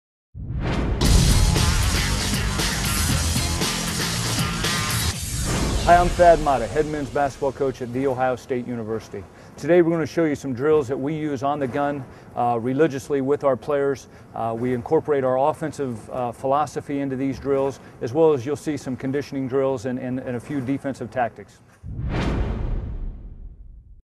Amazing Basketball Shooting Machine - The Gun - Thad Matta
Thad Matta shows some drills that he uses at The Ohio State University using the Gun basketball shooting by Shoot-A-Way